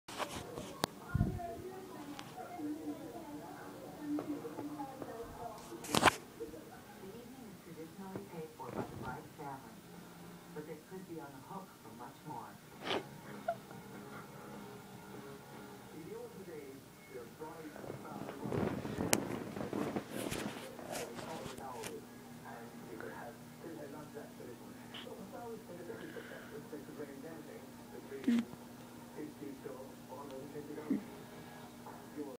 The Night Is Young!!!
It was 11pm and I was watching Friday Night Cranks on my iPod when I noticed Lauren wasn't talking. I looked over to the couch to find her knocked out, and I thought, HEY, why not?!